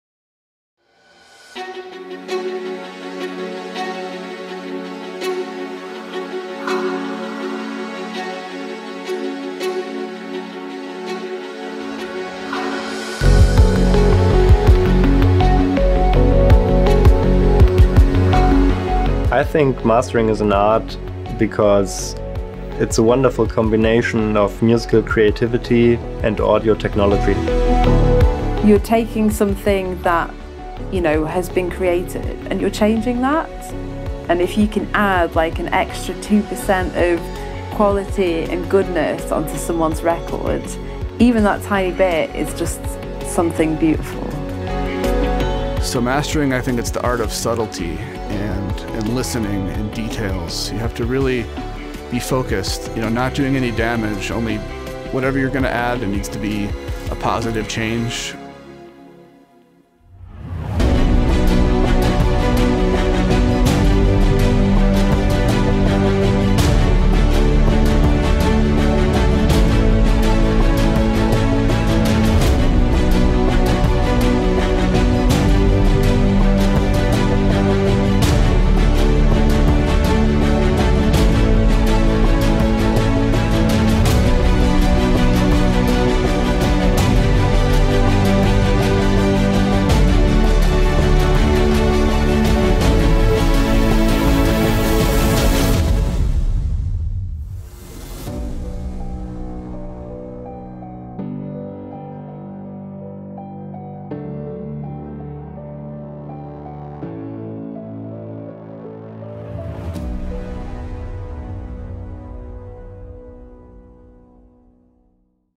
The True Art of Mastering | WaveLab 10 Teaser
WaveLab 10 represents the true art of mastering with a range of improvements and new features for editing and mastering audio. Now including video playback support, extended external effects integration, reference track, enhanced undo/redo and more, the new features in WaveLab 10 further cement its place  as the best mastering software for Mac and PC in the professional audio industry.